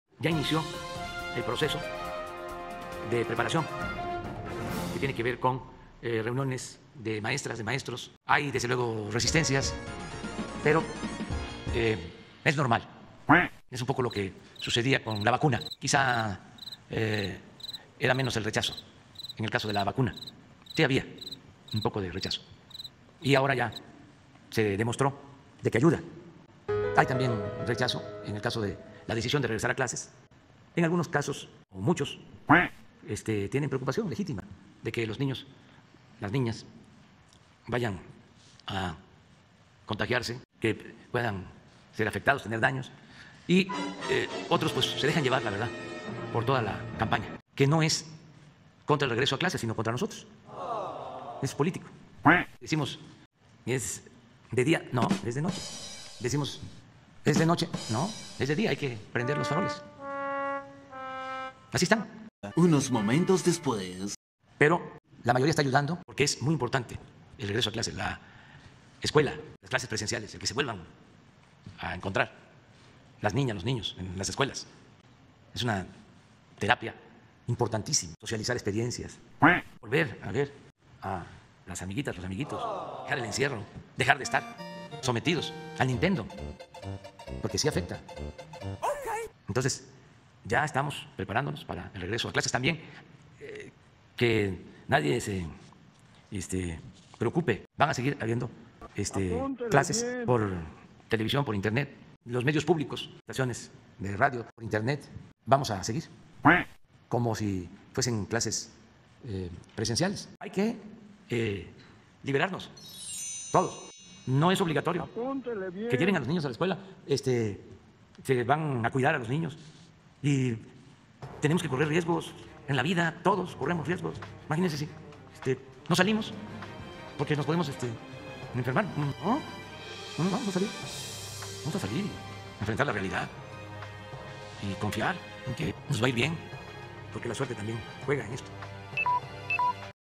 El regreso a clases presenciales es un poco como el rechazo a la vacuna, tenemos que correr riesgos
El regreso a clases presenciales es un poco como el rechazo a la vacuna, es un asunto político en contra de nuestro gobierno 🦆, tenemos que correr riesgos, no podemos vivir con miedo, hay que liberarnos y confiar en que nos va a ir bien 🦆